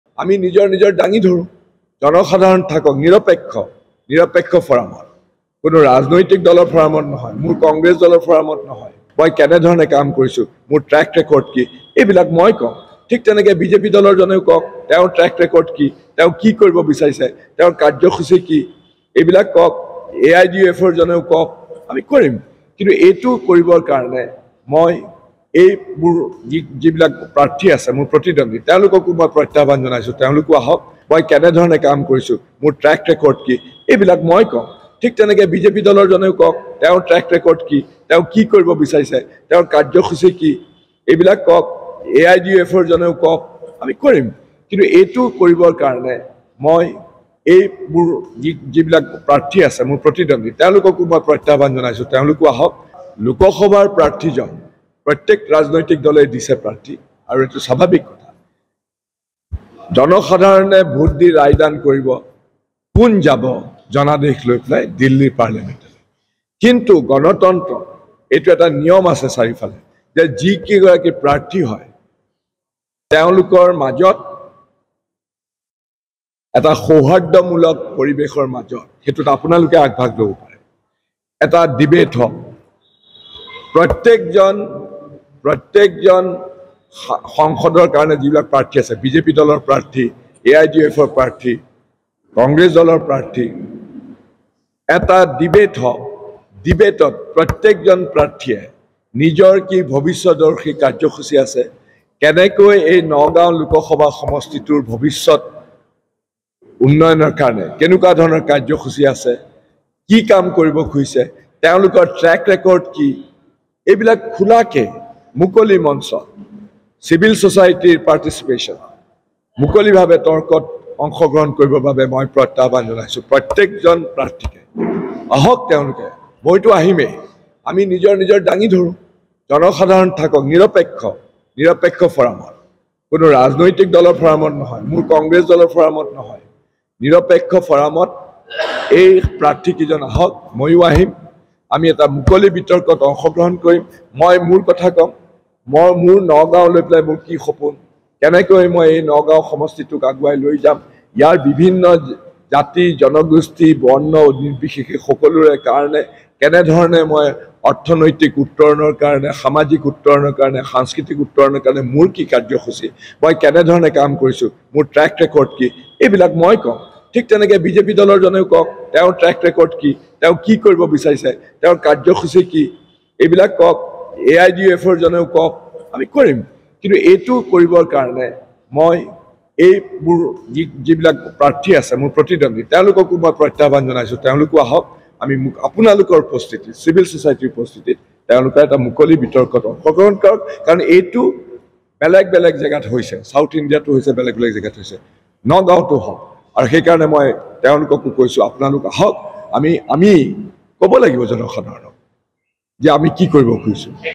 Pradyut Bordoloi News : বিজেপিয়ে অপৰাধীক প্ৰশ্ৰয় দিয়াৰ অভিযোগ প্ৰদ্যুৎ বৰদলৈৰ
Pradyut Bordoloi News | বিজেপিক সমালোচনা ৰকিবুল-প্ৰদ্যুতৰ।
নগাঁও জিলা ৰাজীৱ ভৱনত সংবাদমেল পাতি সমালোচনা।
"সুৰেশ বৰাই দিনে ৫০ লাখ উপাৰ্জন নকৰে।
তেঁও শপতনামাত এই বিষয়ে দেখুৱাব পৰা নাই।
মোৰ আৰু নুৰুল হুদাৰ গাড়ী ভঙা অপৰাধীক প্ৰশ্ৰয় দিছে বিজেপিয়ে।"
এই অভিযোগ ৰকিবুল হুছেইনৰ।
নগাঁও লোকসভাৰ ৰাইজক বিবেক ভোট দিবলৈ আহ্বান ৰকিবুল হুছেইনৰ।
প্ৰদ্যুৎ বৰদলৈয়ে বিচাৰিলে মুকলি বিতৰ্ক।
বিজেপিয়ে অপৰাধীক প্ৰশ্ৰয় দিয়াৰো অভিযোগ বৰদলৈৰ